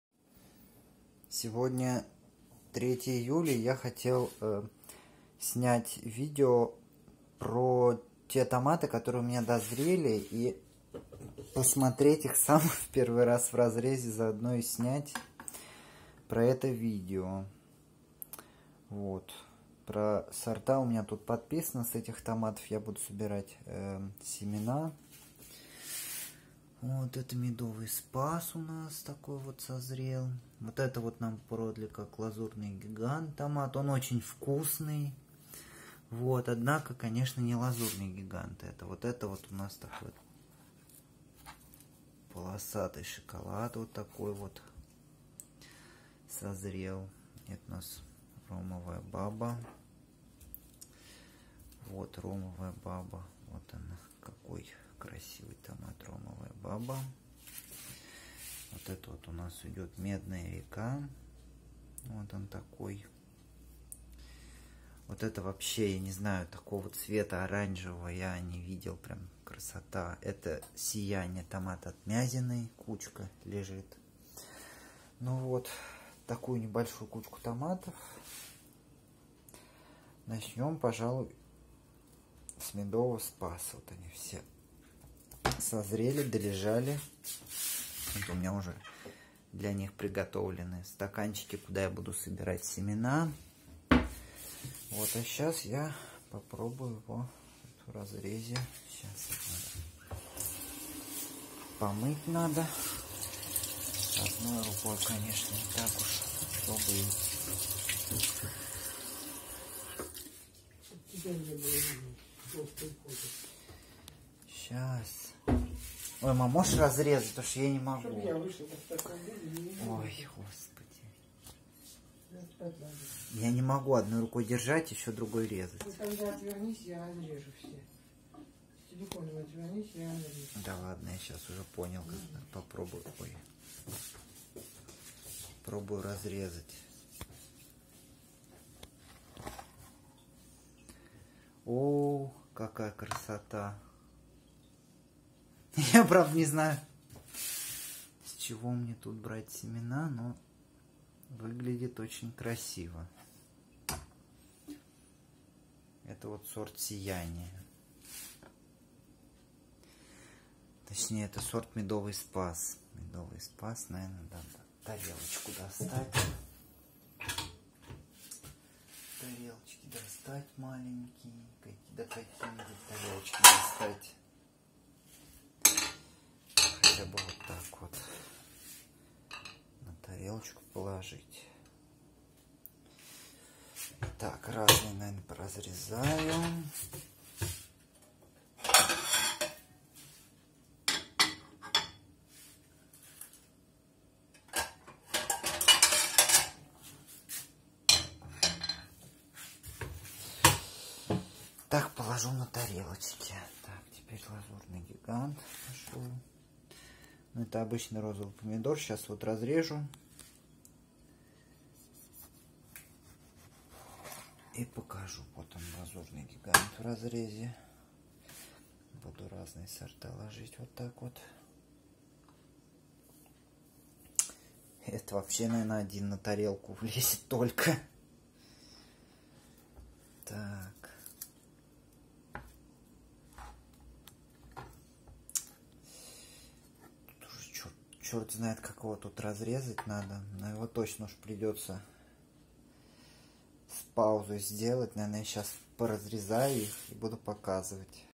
Томаты выбранные на семена. Часть 1
Собрал некоторые томаты на семена,  но разрезать одной  рукой с телефоном было неудобно и пришлось разбить его на 2 части. Сорта:
Ромовая баба
Полосатый шоколад
Сияние
Лазурный гигант (пересорт)
Медовый спас
Copper river